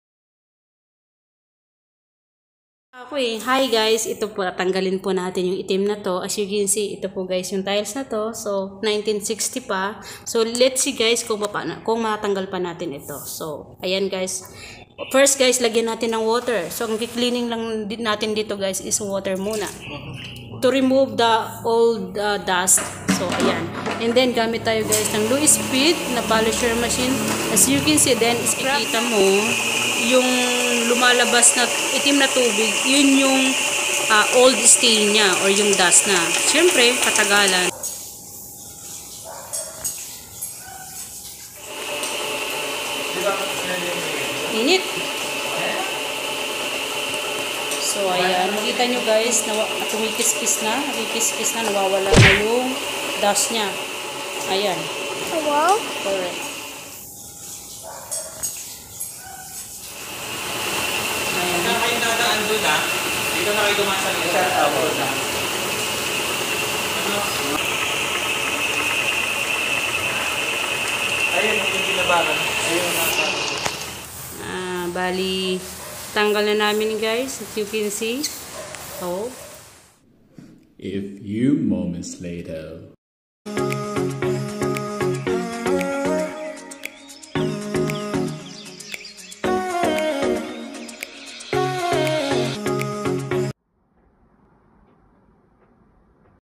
How to strip terrazo floor?|Cleaner Rosie
This video shows how it cleaned.. So watched and learned..

Pls support my cleaning channel..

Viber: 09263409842